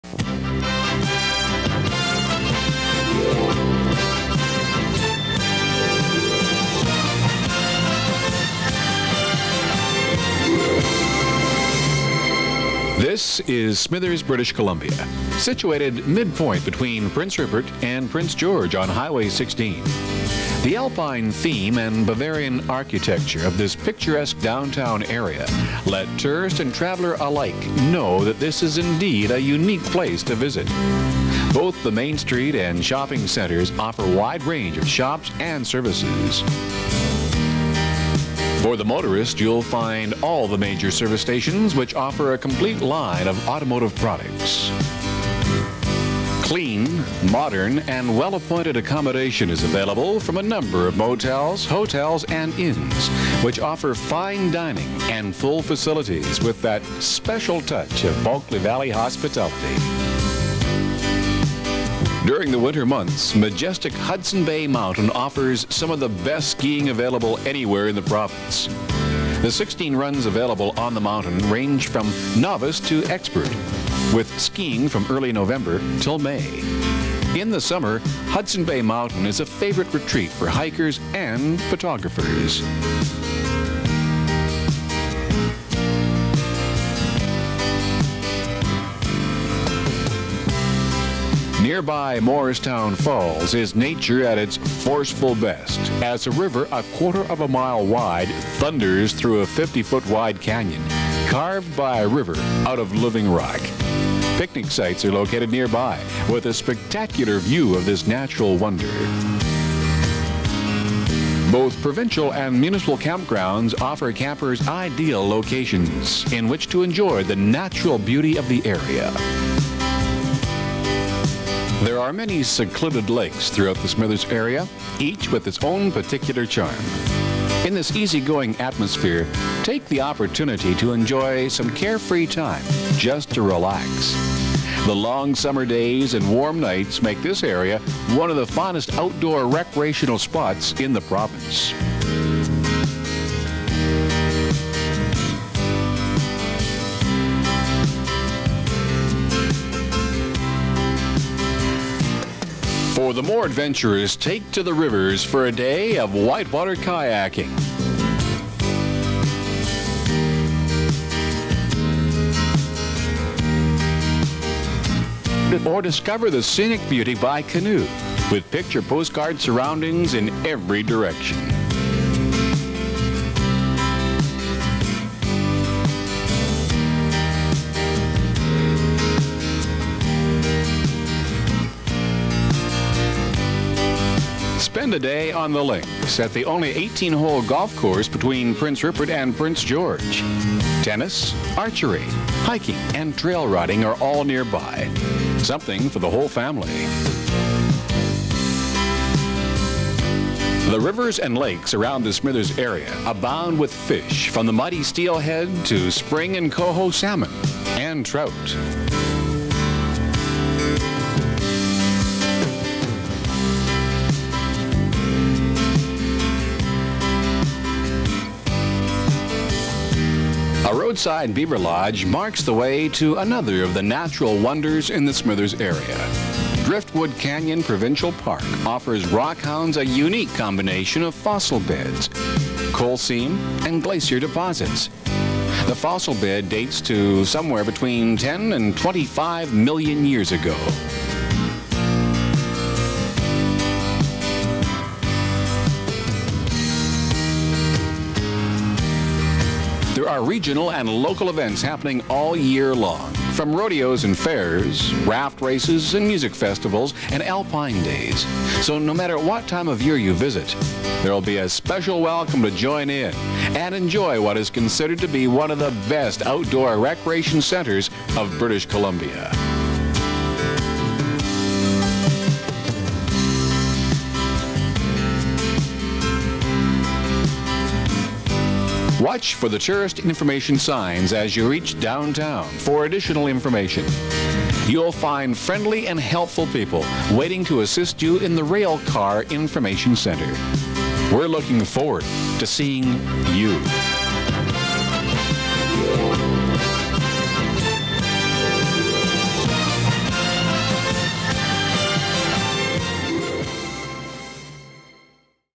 Smithers BC Promo Video - A Blast from the Past
This is a little video made back about 25 years ago to promote tourism in the Bulkley Valley. Step Back in time and have a laugh and see the surrounding area compared to today, my how times change. Enjoy!